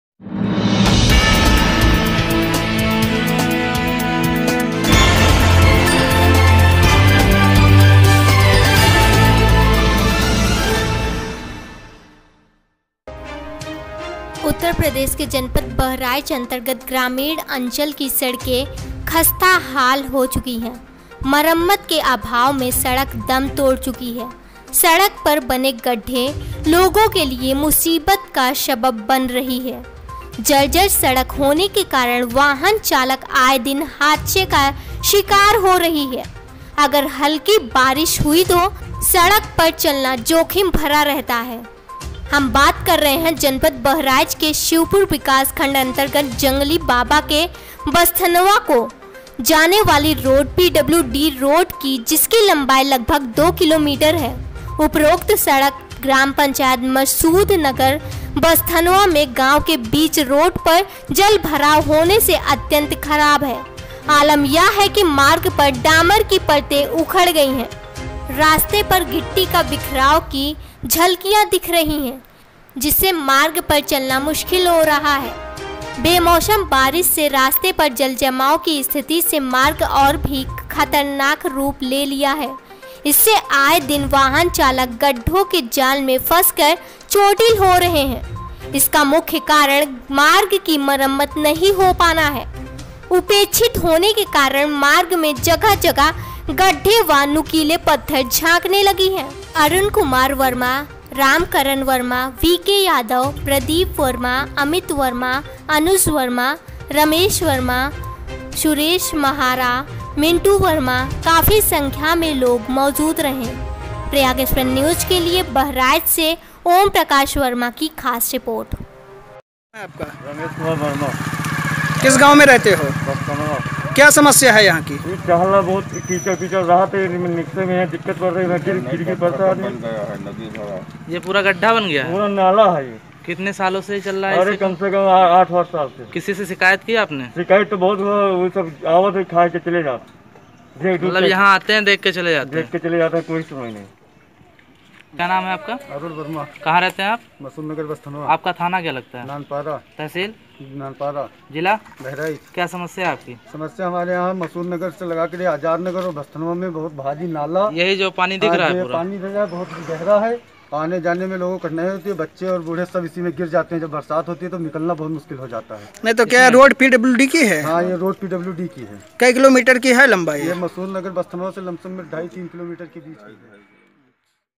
उत्तर प्रदेश के जनपद बहराइच अंतर्गत ग्रामीण अंचल की सड़कें हुई खस्ताहाल
प्रयाग एक्सप्रेस न्यूज़ रिपोर्ट जोनल हेड ओम प्रकाश वर्मा